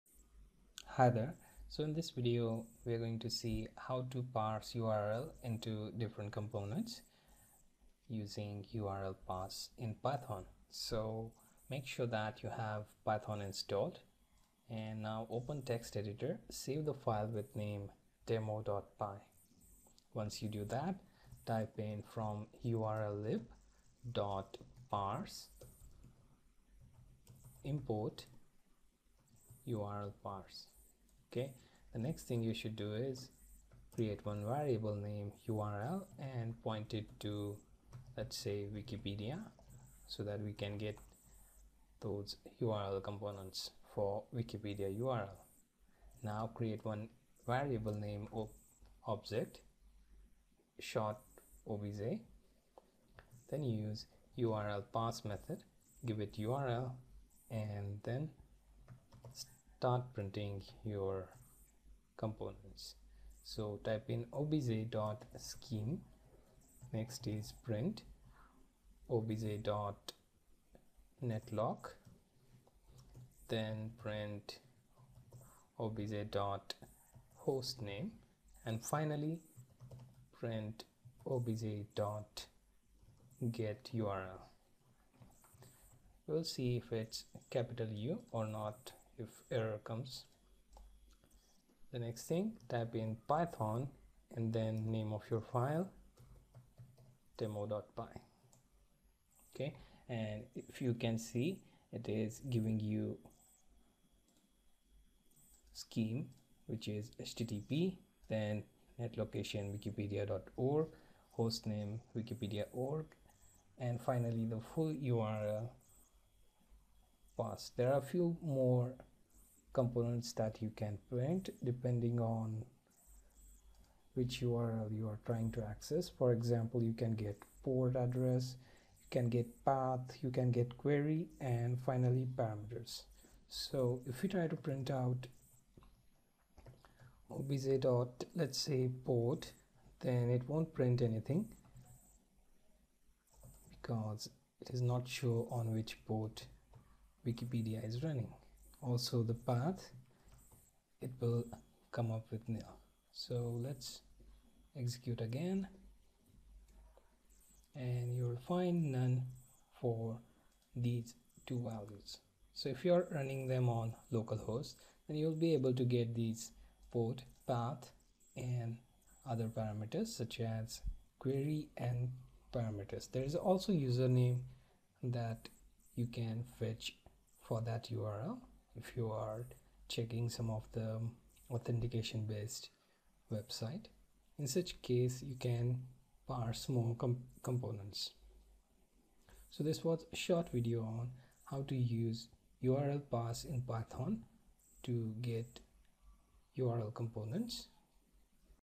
Python URL Parse into Components
Learn how to parse URL into small components using urllib.parse.